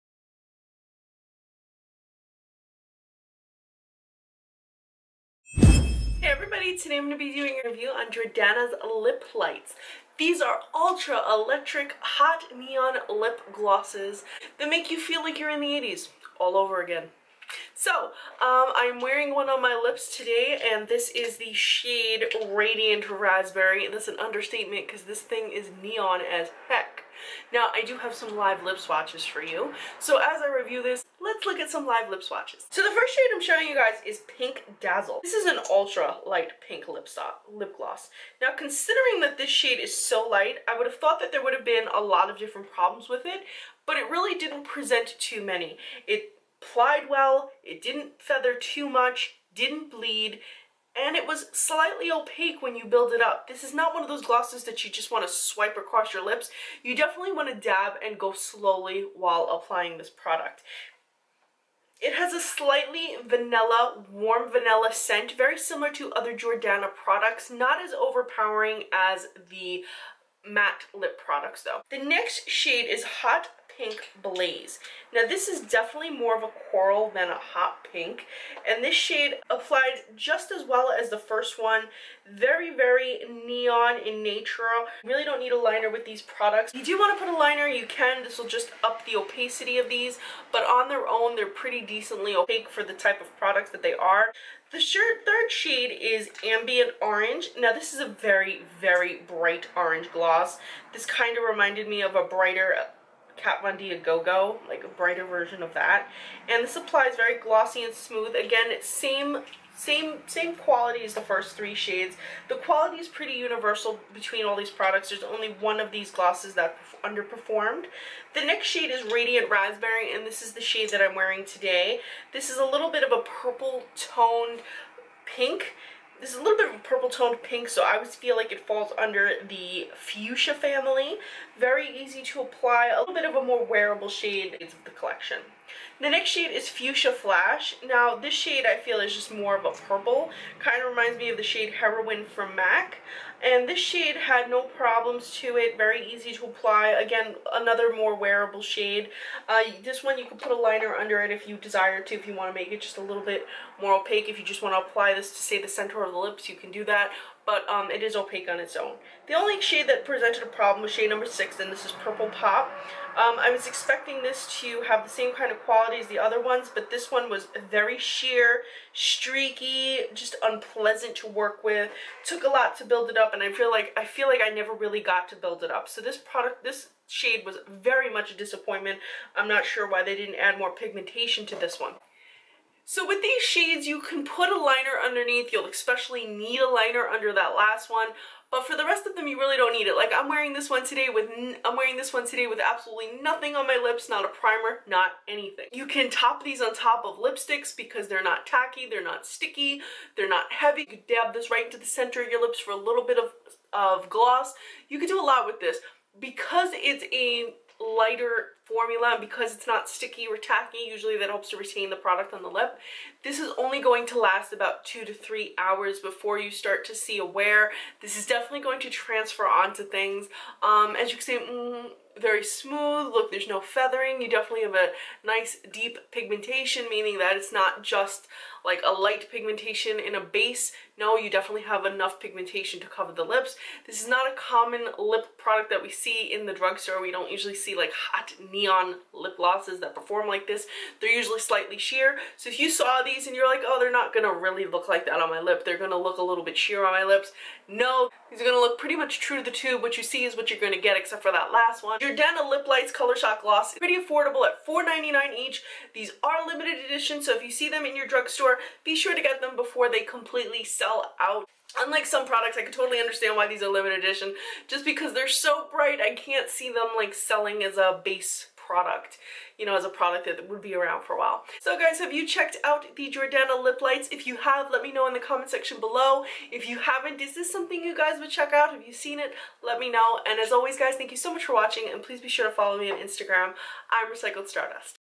Jordana Lip Lights Colorshock Gloss | Review + Swatches
The Jordana Lip Lights Colorshock gloss is available on the Jordana website for 4.99$ each!